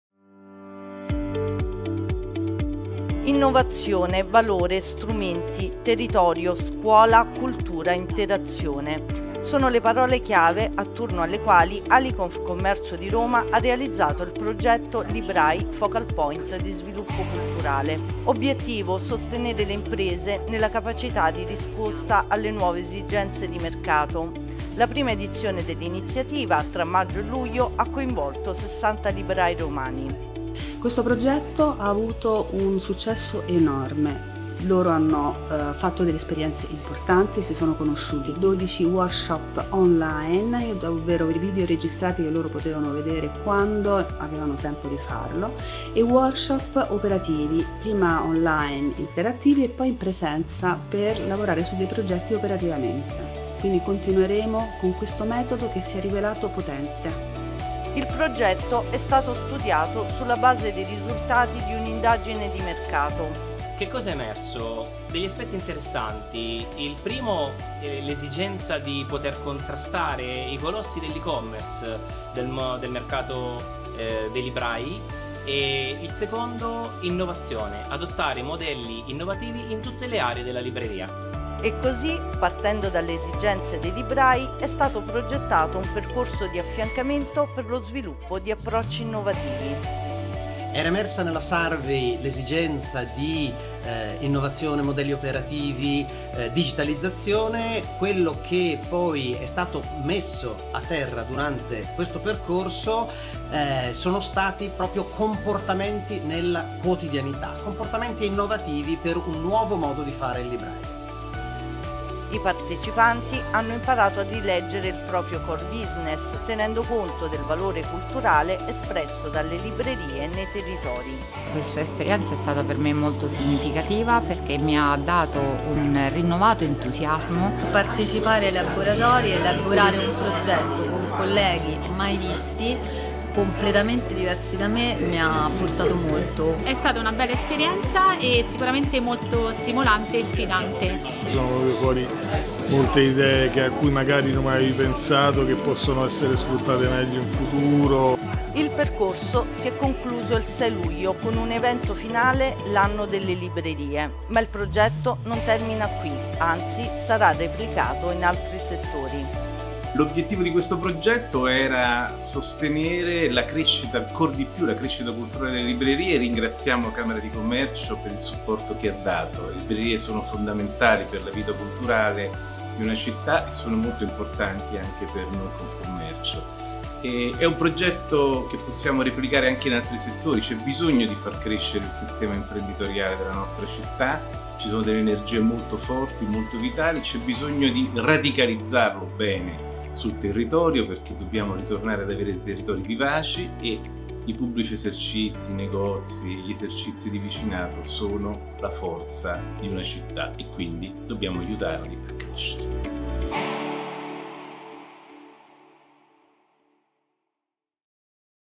“L’anno delle librerie”: il progetto di ALI Confcommercio Roma
Si è appena conclusa la prima edizione dell’iniziativa “L’anno delle librerie” di ALI Confcommercio Roma, partita a maggio, che  ha coinvolto 60 librai romani.

Innovazione, valore, strumenti, territorio, scuola, cultura, interazione: sono le parole chiave attorno alle quali ALI Confcommercio Roma ha realizzato il progetto “Librai: focal point di sviluppo culturale”.